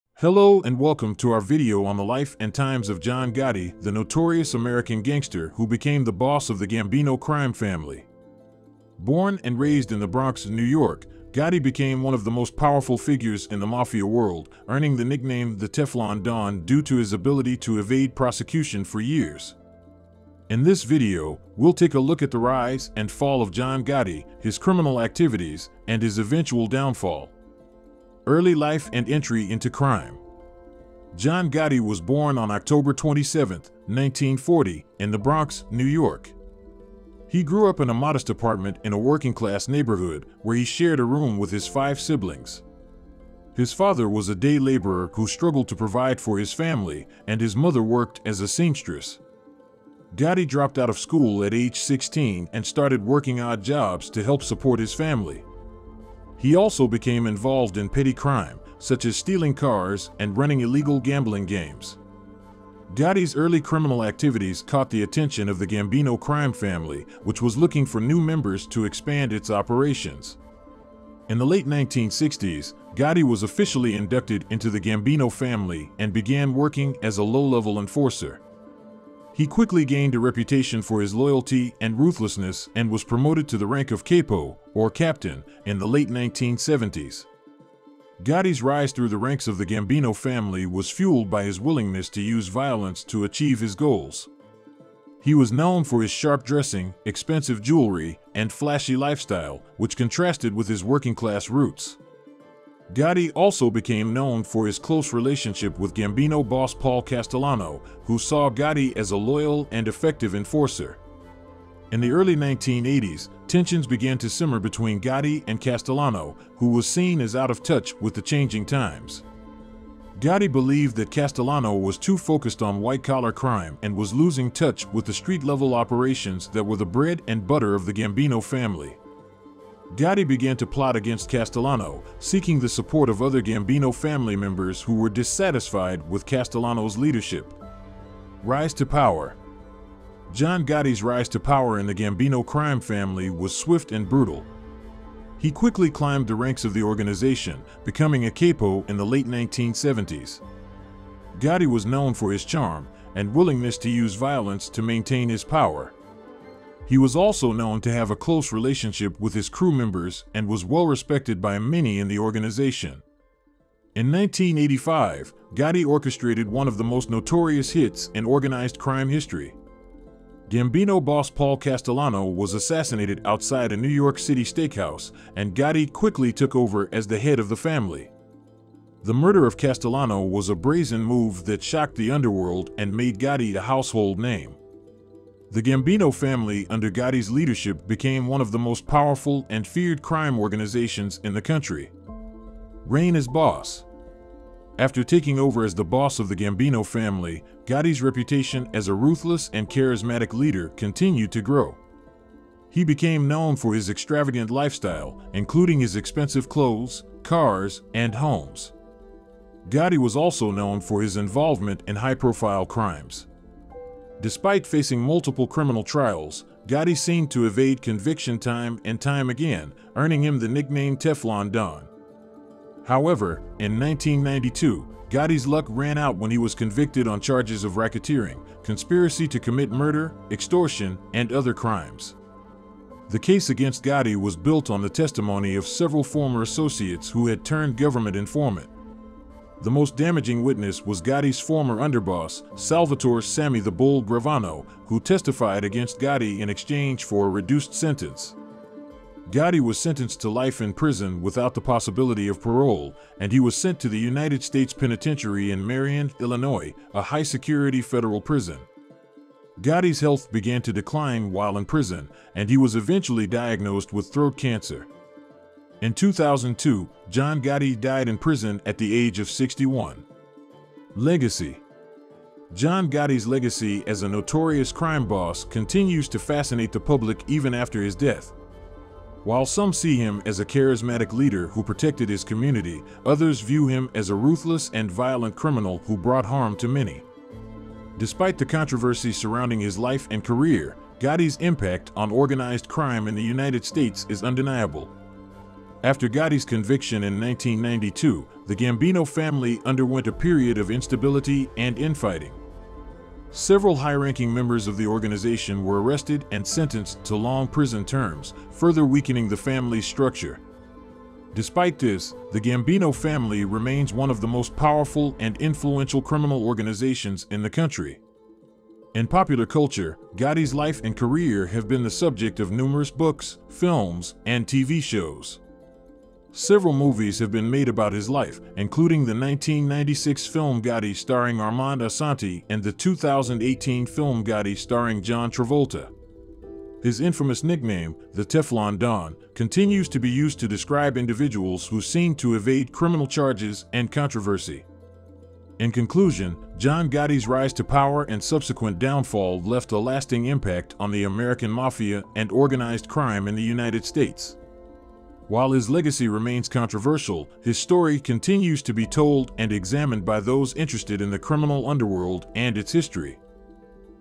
John Gotti - Biography of the boss of Gambino family
John Gotti - Biography of the famous gangster and the boss of Gambino family